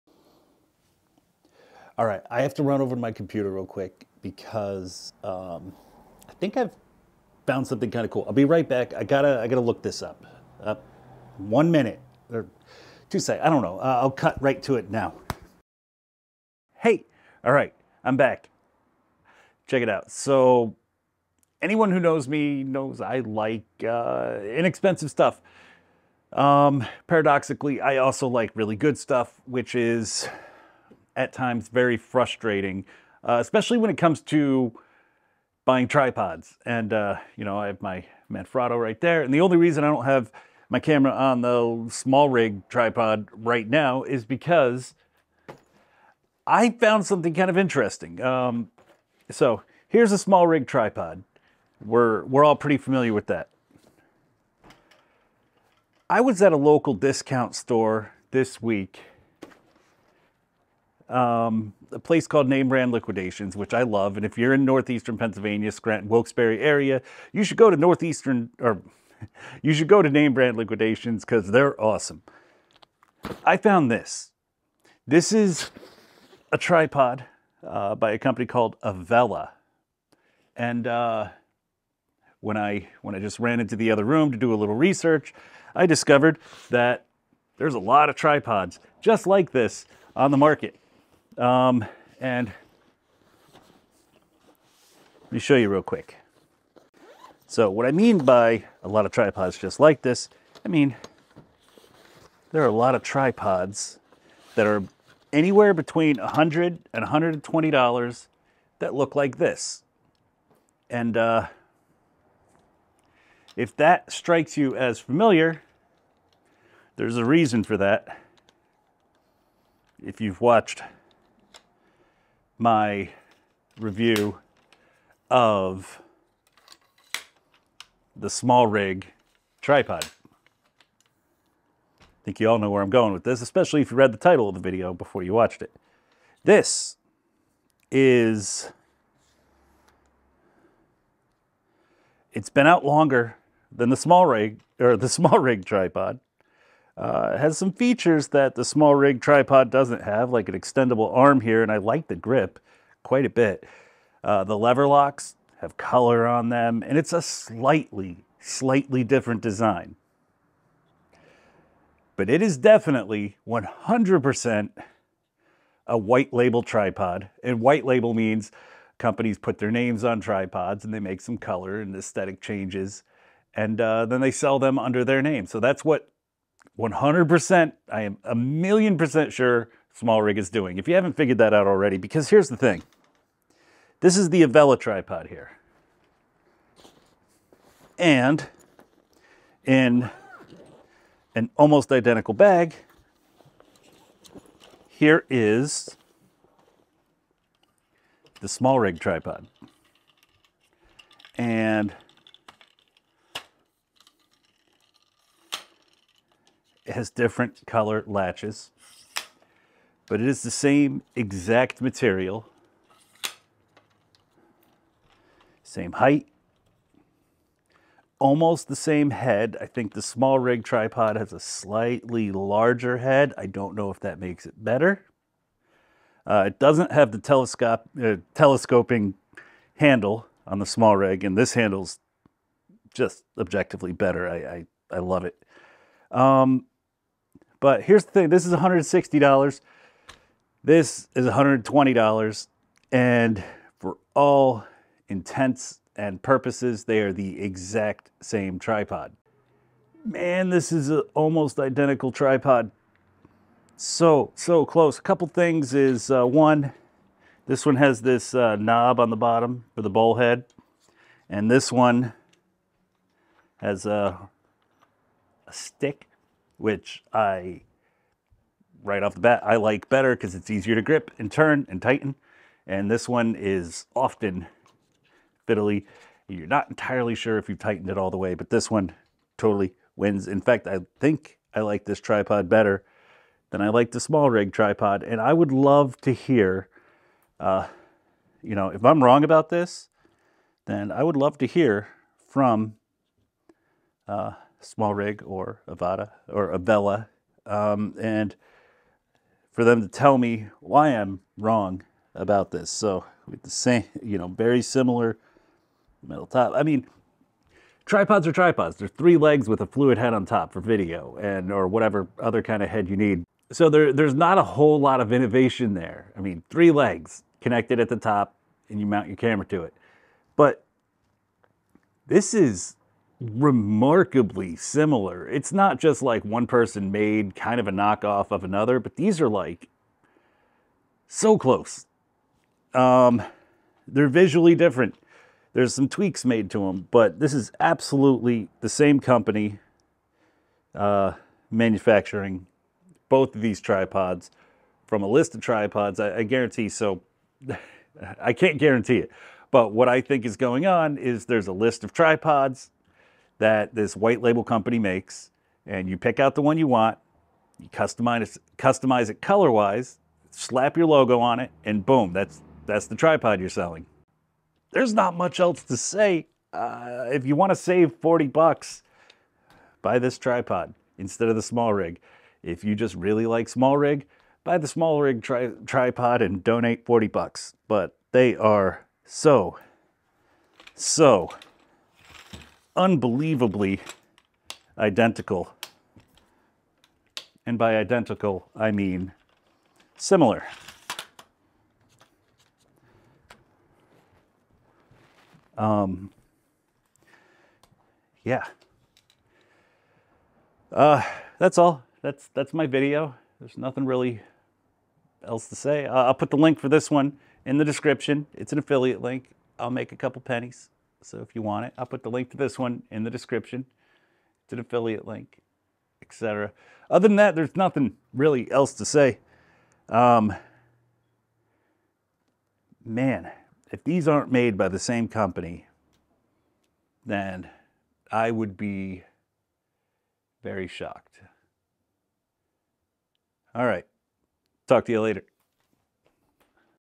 Is SmallRig White-Labeling their Tripods?
Is SmallRig white-labeling their new line of tripods?

Avella Fluid Head Tripod
Smallrig Fluid Head Tripod